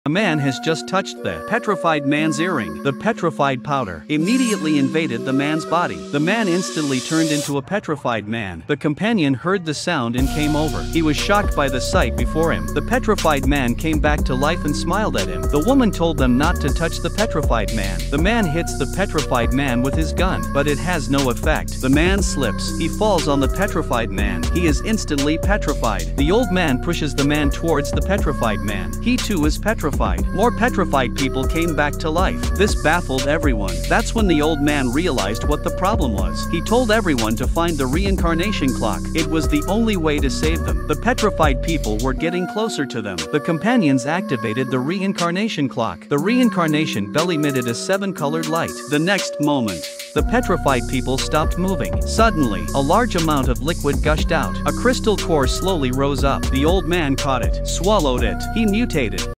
The man touched the petrified man's earring and instantly became a petrified man
Yi Sa trained Zong Hang to become a professional mandrill, and together with the Ding family mandrill, Ding Yudie, the three of them went into the water to enter the "Ancestral Source Golden Soup". Ding Changsheng, the head of the Ding family, also covets the "water spirit gall" in order to order the three great families. In the so-called "Asian Bermuda" at the bottom of the Palace Pavilion Lake, a fierce competition is about to unfold .